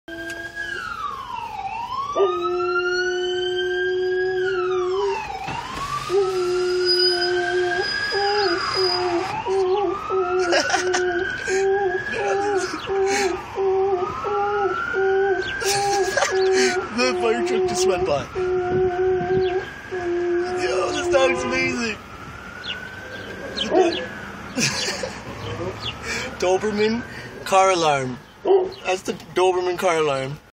Doberman Car Alarm
Doberman reacts to car Siren! Jokes!